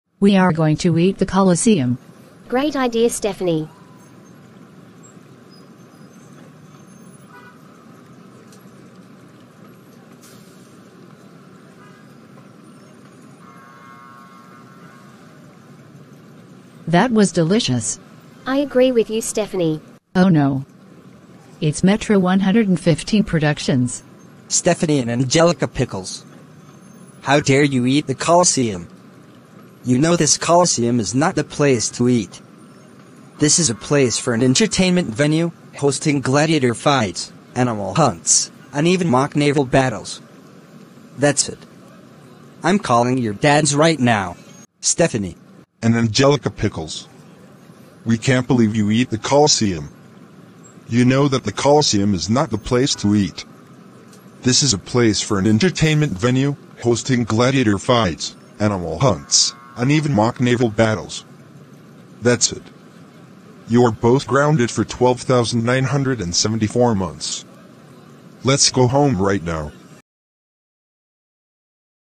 Stephanie And Angelica Pickles Eat The Colosseum And Get Grounded
Bar on the run